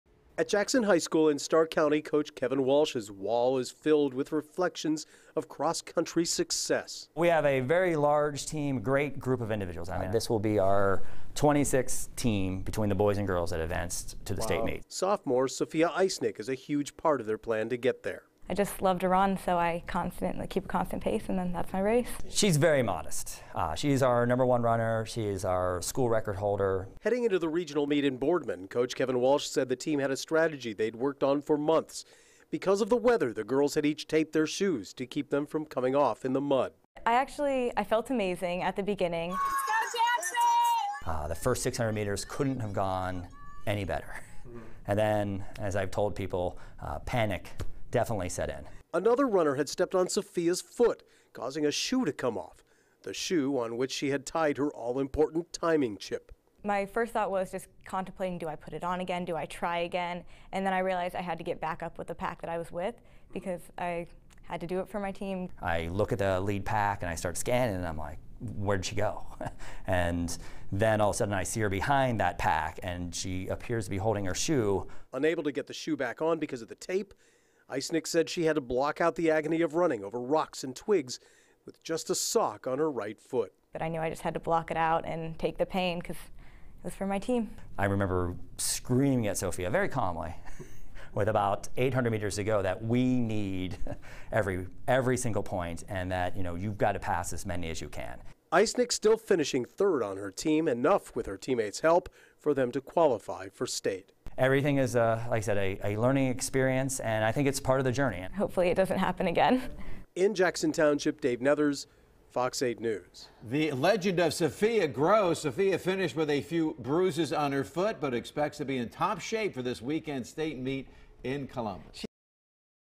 High School Runner Finishes Race Without Shoe
A sophomore cross country runner at Jackson High School was able to finish an important race with only one shoe on.